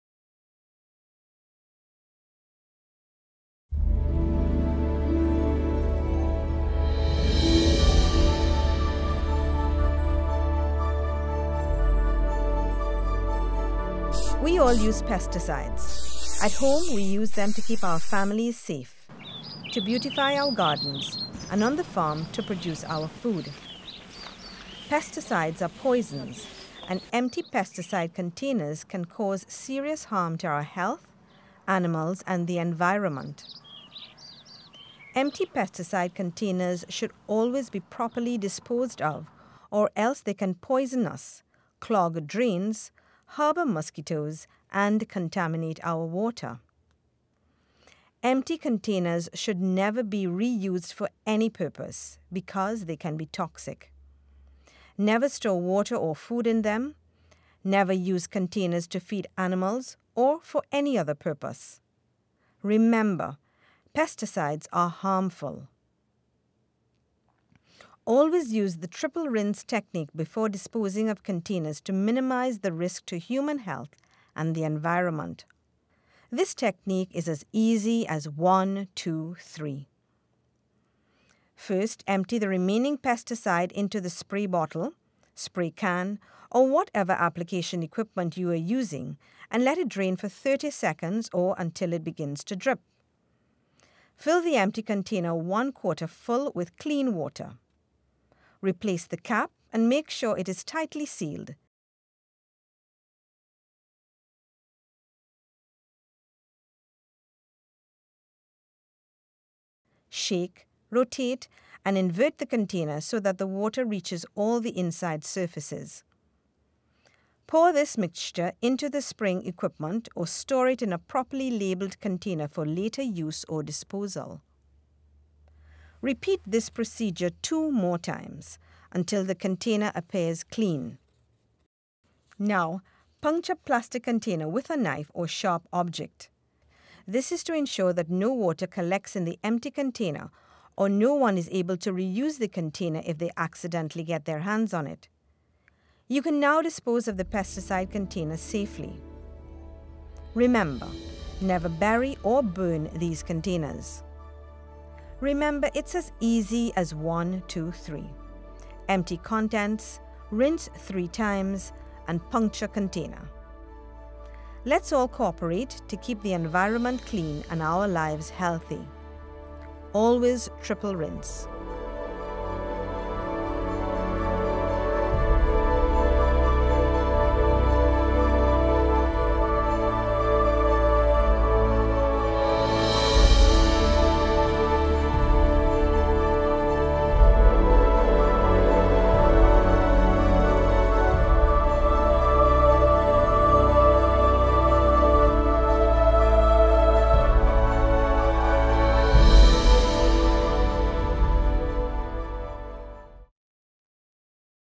Triple Rinse Technique
Pesticides forms part of the everyday lives of some Agricultural producers, a question that is frequently asked would be, how do i dispose of these containers after i would have finish using them?. This infomercial gives an insight into the triple rinse technique of disposing empty pesticide containers.